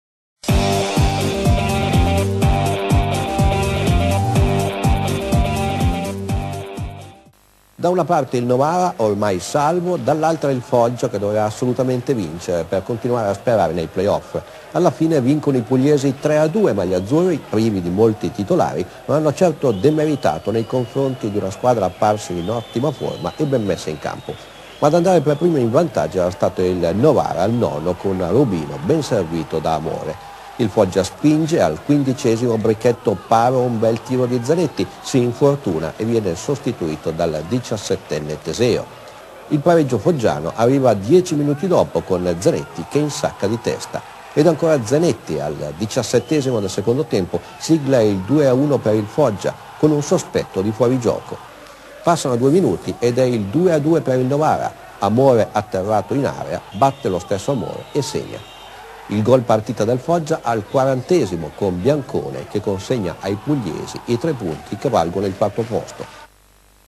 Novara-Foggia 2-3 (32ª giornata 2007/08)
Serie C1/A 2007/08 - 32^ giornata 20/04/2008.
Alle reti di Rubino e Amore del Novara risponde il Foggia con la doppietta di Zanetti e il gol sul finale di gara di Biancone.